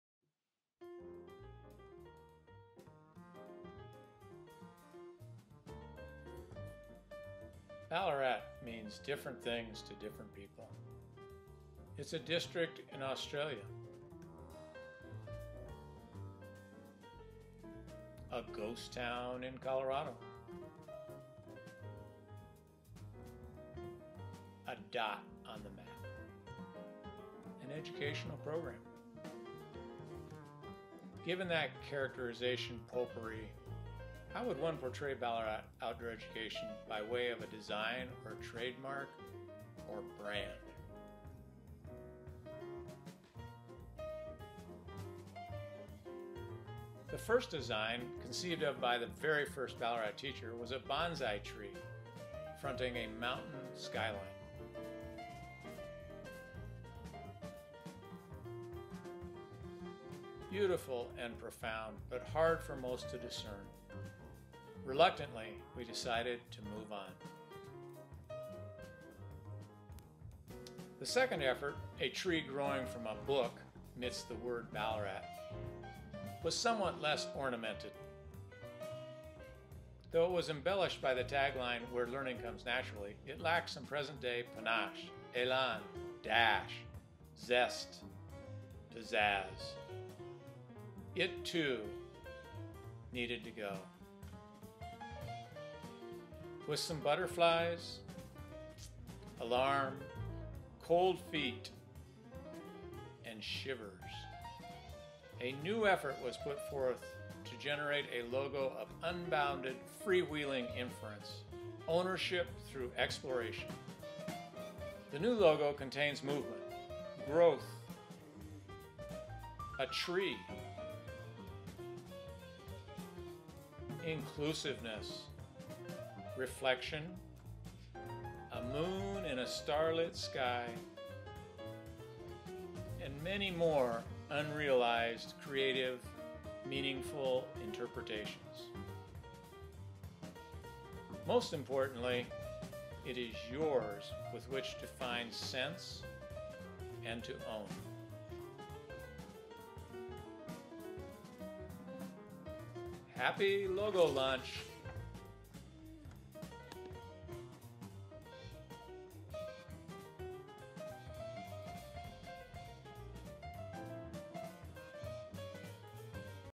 Balarat Outdoor Education Branding
How would one portray Balarat by way of design, trademark, or brand?  Learn how Balarat got a new logo from the team of Land & Sea Co. by this creative video made by Patrick & Elle Emery.